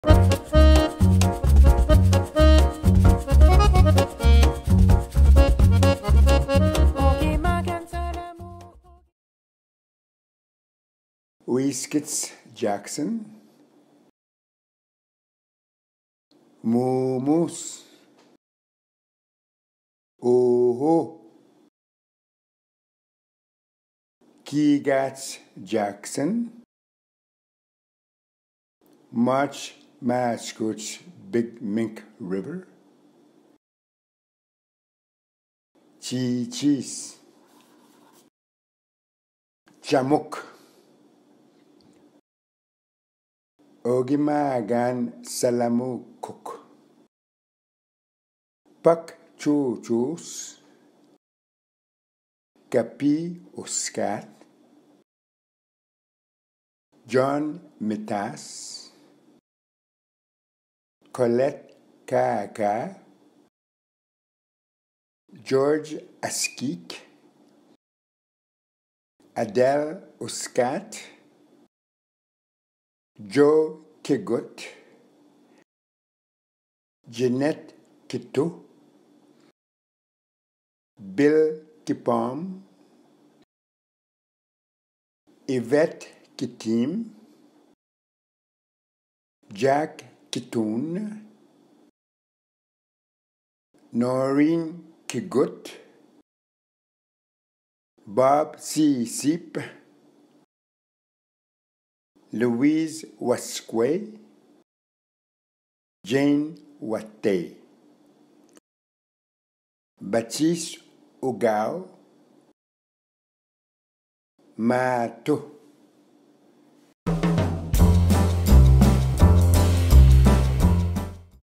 Learn how to say the Cree character names in "Grand Chief Salamoo Cook Is Coming to Town!"
Listen as Tomson Highway demonstrates how to pronounce the Cree character names in his musical picture book "Grand Chief Salamoo Cook Is Coming to Town!"

Grand Chief Salamoo Cook Is Coming to Town!
Available September 2023
Story and Songs Tomson Highway
Illustrations Delphine Renon
Narration Jimmy Blais
Singers Coral Egan, Alexandre Désilets, Moe Clark and Angel Baribeau

To order books for your library or bookstore, please contact Independent Publishers Group (United States), Hornblower Books/University of Toronto Press (Canada), and MMS/BookSource (United Kingdom).